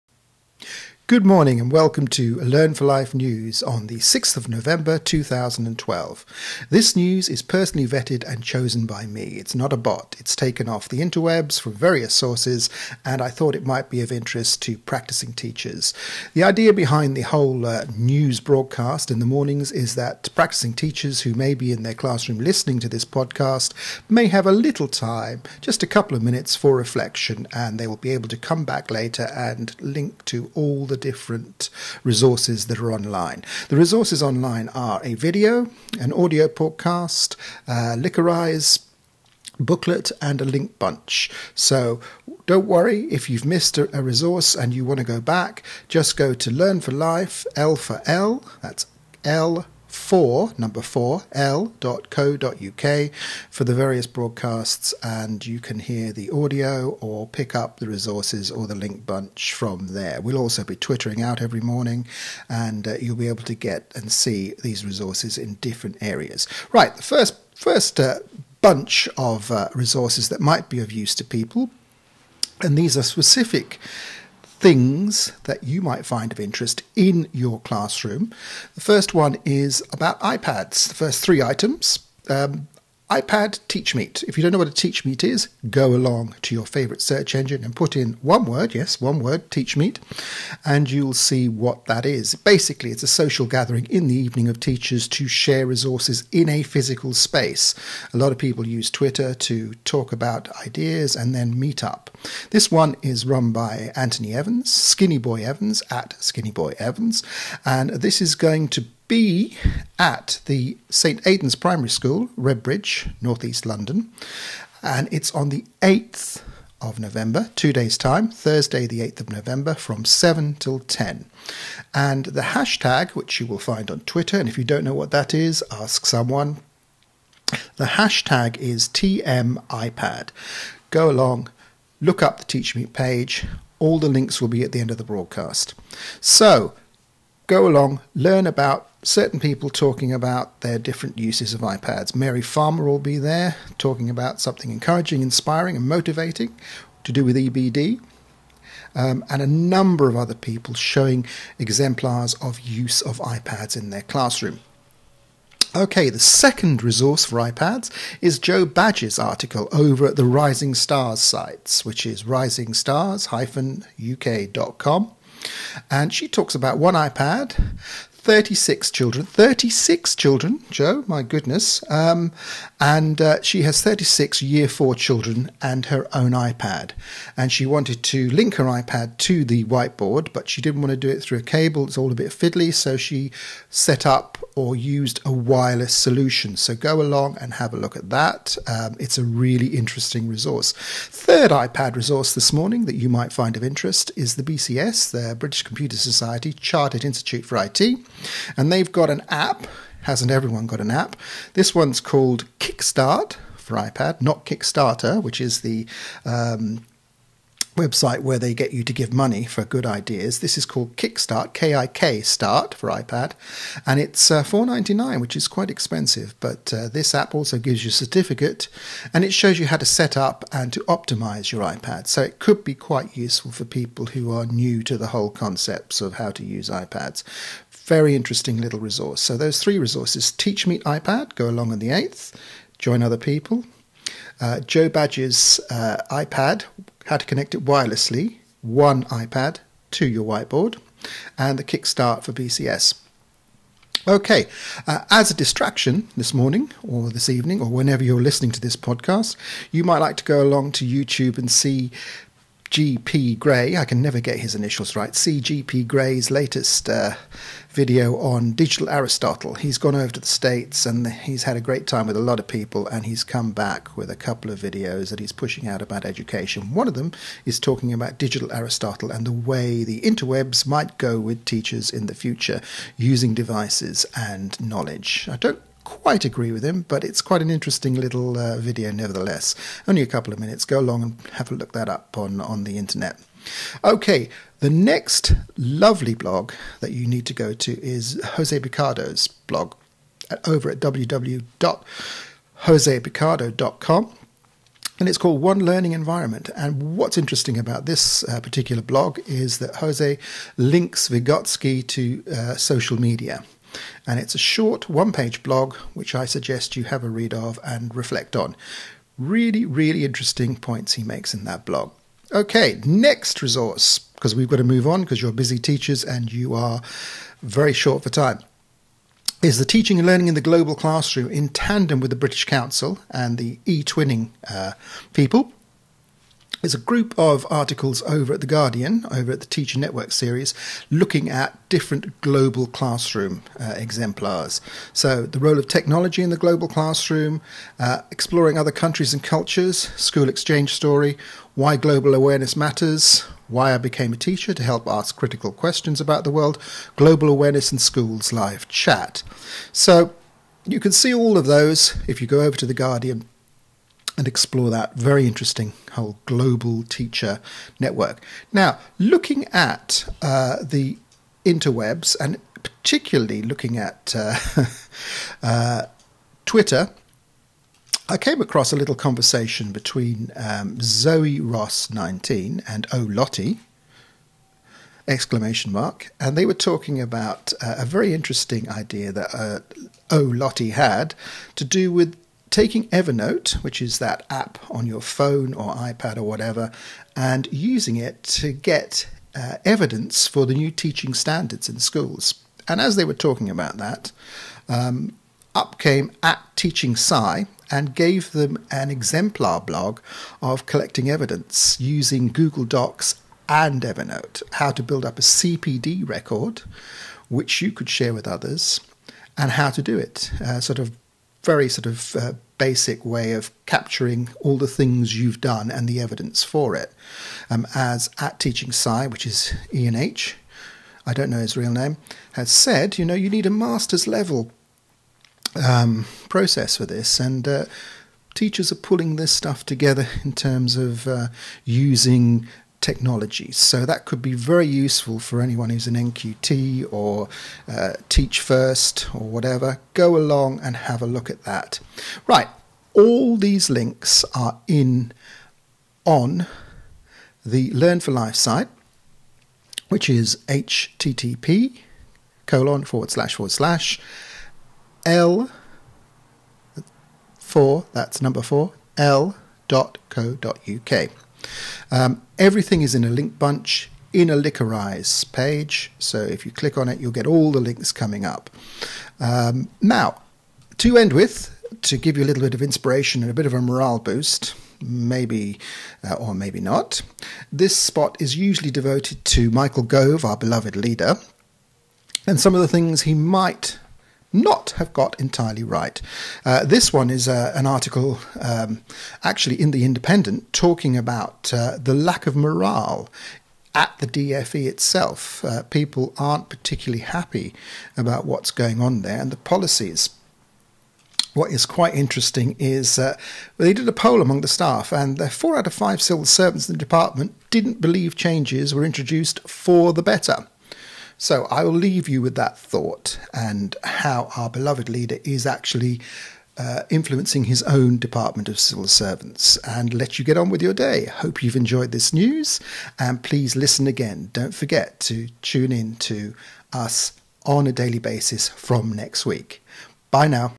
Learn 4 Life News 06-11-12
Index

iPads R Us

0:48 - Address of blog
1:20 - TeachMeet iPad
2:35 - @jobadge
3:10 - BCS Kikstart

Educational Theory

4:00 - CGP Grey's Digital Aristotle
4:44 - José Picardo on Vygotsky and Social Media

Global Education

5:10 - Guardian Teacher network

Bright Ideas

5:57 - Using Evernote for CPD and Teaching Standards

Links to resources in one place

7:20 - Links for all resources

Michael Gove Spot

7:50 - Low morale at the DfE